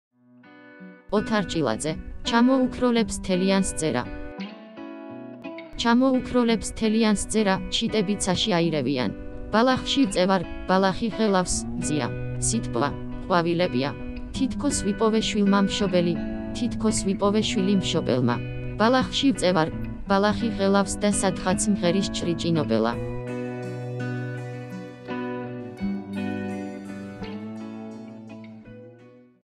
#83 ოთარ ჭილაძე - ჩამოუქროლებს თელიანს ძერა. კითხულობს "რობოტი"
ოთარ ჭილაძე - ჩამოუქროლებს თელიანს ძერა.
კითხულობს "რობოტი".

ჩამოუქროლებს თელიანს ძერა,
ჩიტები ცაში აირევიან.
ბალახში ვწევარ,
ბალახი ღელავს,
მზეა... სითბოა... ყვავილებია...

თითქოს ვიპოვე შვილმა მშობელი,
თითქოს ვიპოვე შვილი მშობელმა...
ბალახში ვწევარ,
ბალახი ღელავს
და სადღაც მღერის ჭრიჭინობელა.